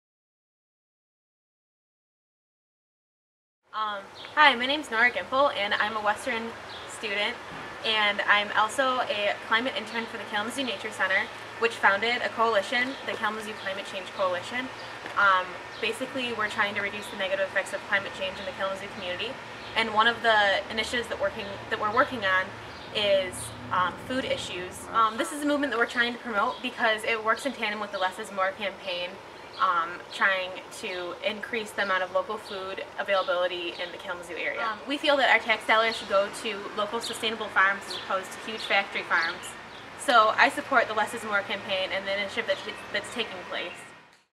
Nora Gimpel Testimonial for Less=More in Kalamazoo, MI
Nora Gimpel is a student at Western Michigan University as well as an Intern for the Kalamazoo Climate Change Coalition through the Kalamazoo Nature Center. Supporting a local food movement is important to her and her work. Check out the full testimonial!